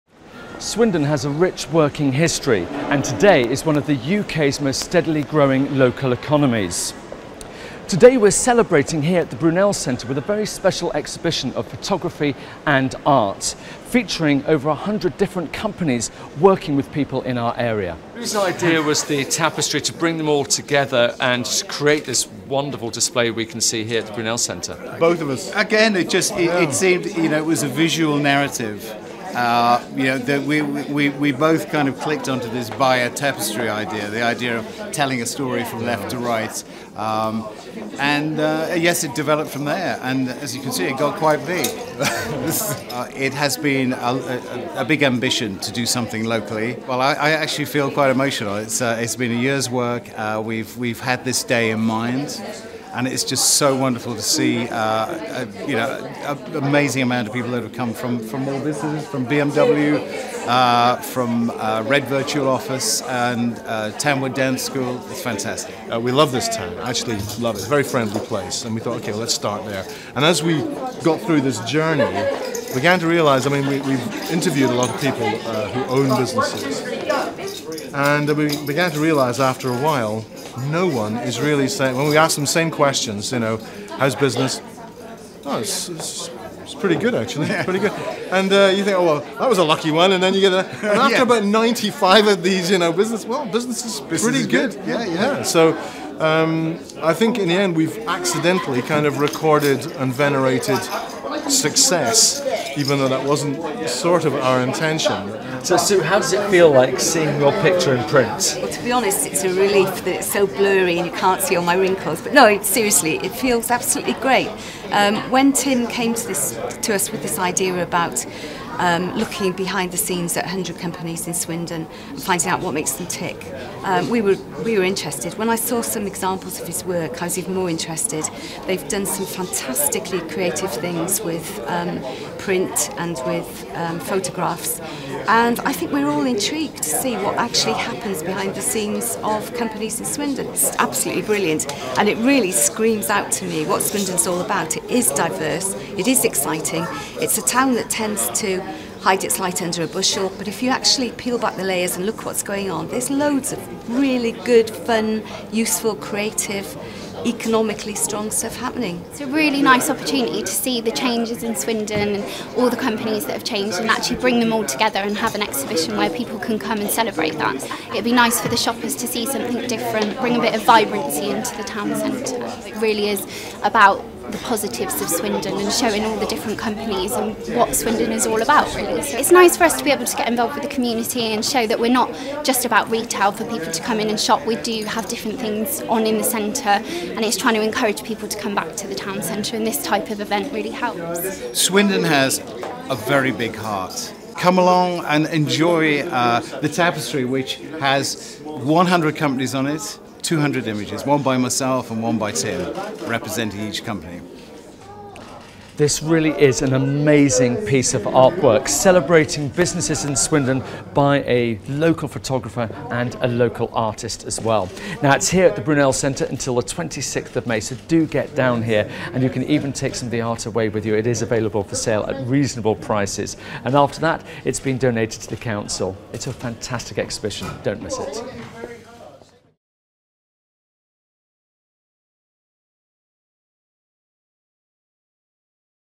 Working Beautifully Digital Paintings at The Brunel Shopping Centre Swindon
Working Beautifully features over six hundred original digital paintings and photographic composites created by Hudson and Cumming over the last twelve months take a glimpse behind the scenes of the some of Swindon's largest and smallest firms.